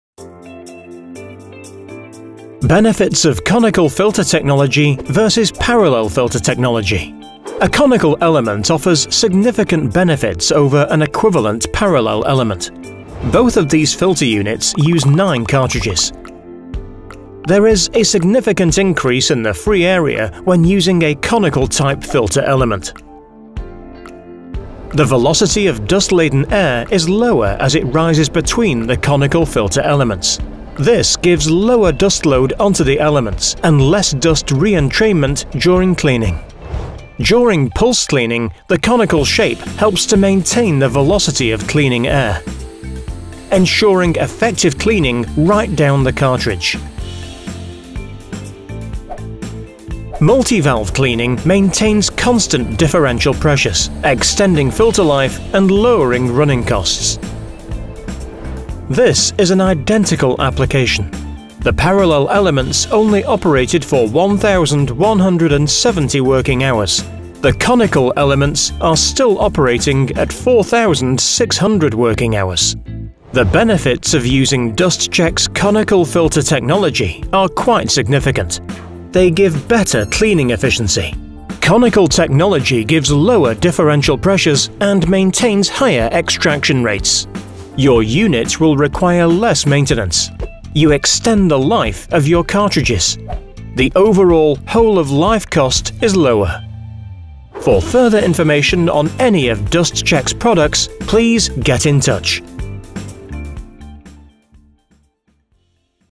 Dustcheck Conical vs Parallel Filter Elements
Why are our Dustcheck dust filter cartridges conical in shape?

• They give better cleaning efficiency
• You can maintain low differential pressure and maintain extraction rates
• Your unit will require less maintenance
• You extend the life of your cartridges
• The overall ‘whole of life’ cost is lower

Watch our one-minute animation and find out how they work.